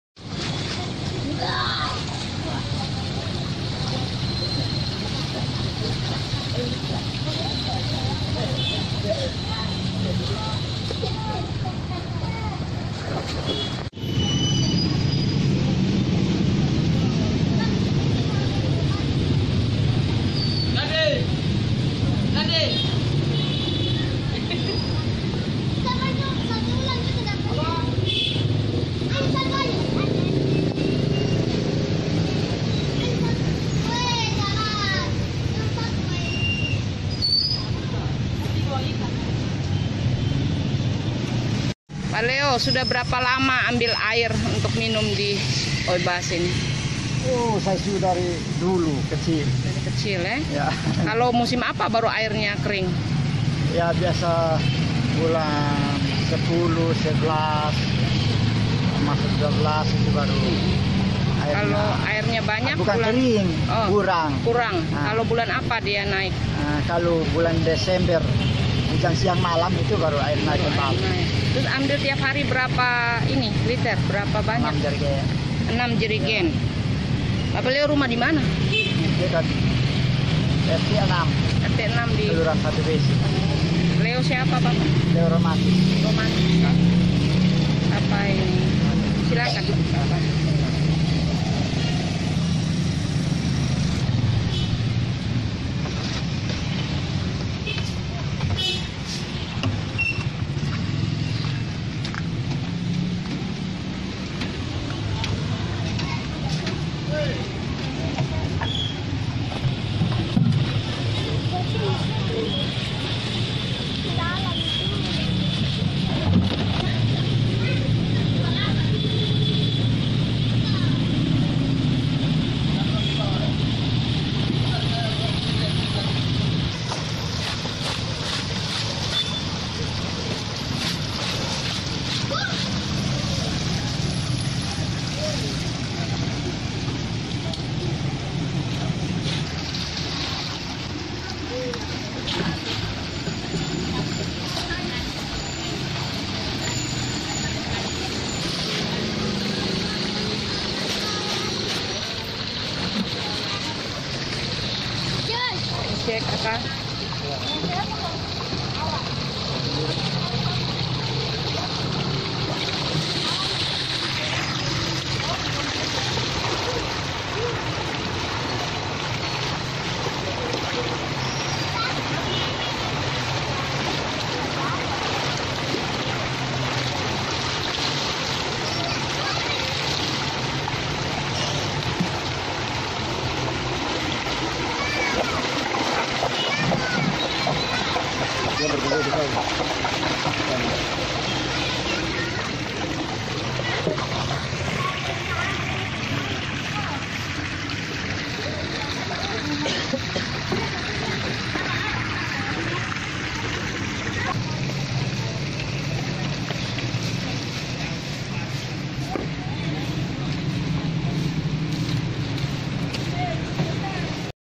Masyarakat Kota Kupang manfaatkan Air Oeba di Kupang 2017
Masyarakat Kota Kupang manfaatkan Air Oeba di Kota Kupang untuk kebutuhan sehari-hari, makan, minum, mandi dan mencuci, Minggu (19/2/2017) pagi.